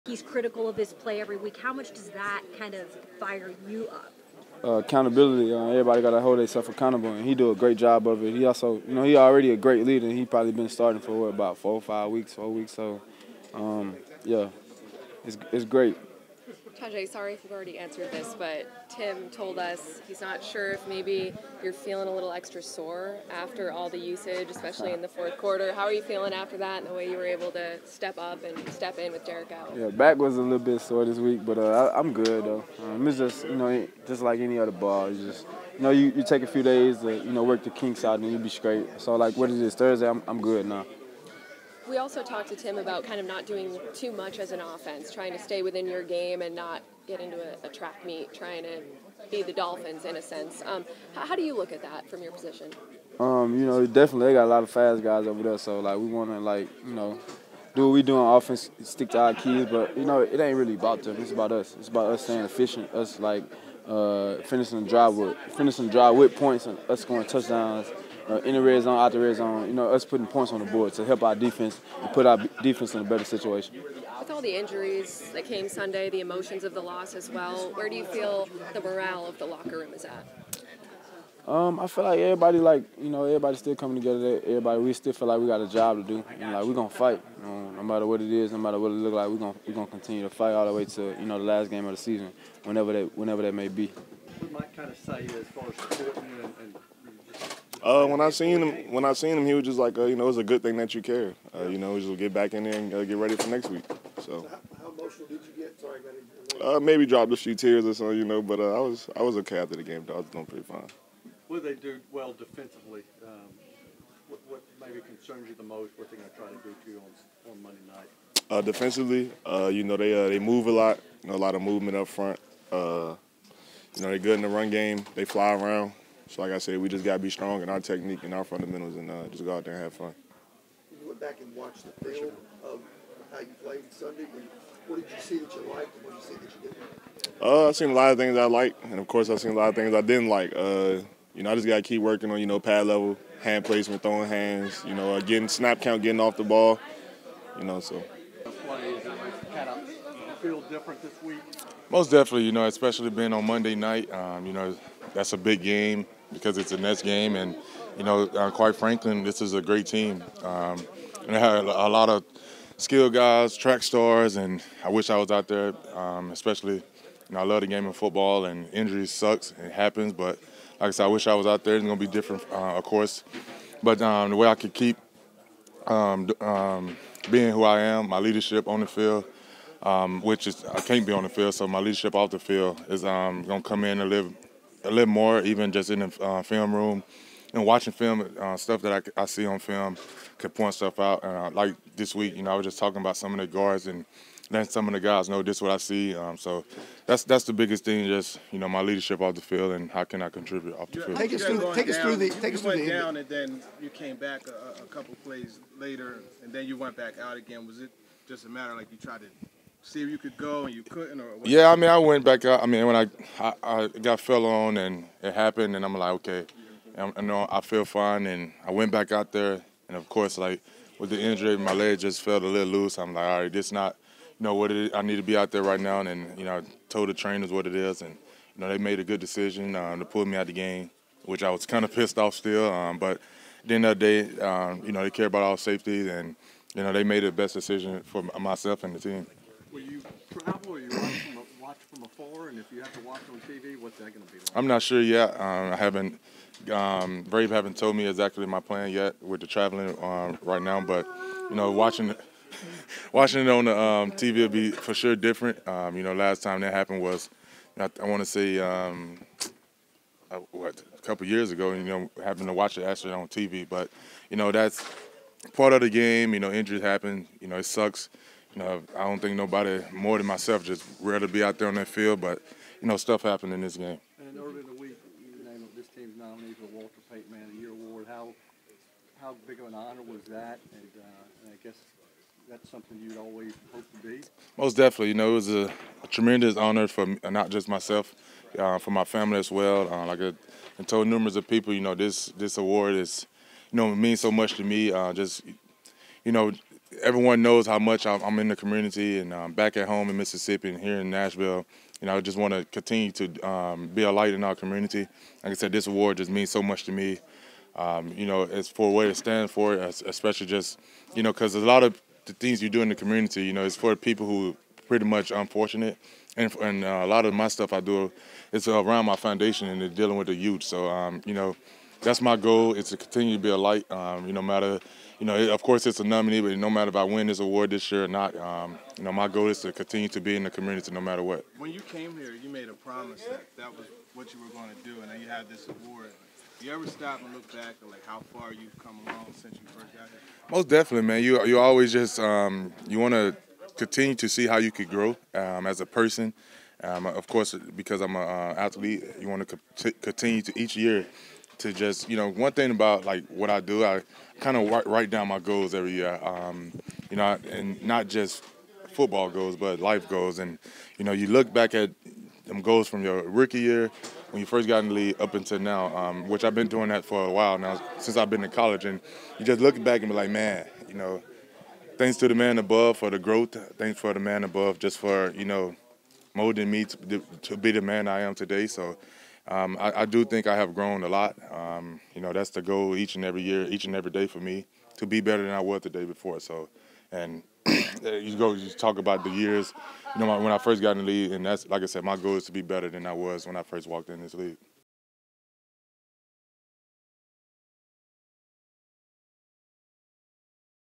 We Still Got a Job to Do | Media Availability
00:00 - RB Tyjae Spears
1:35 - OT Jaelyn Duncan
2:29 - DT Jeff Simmons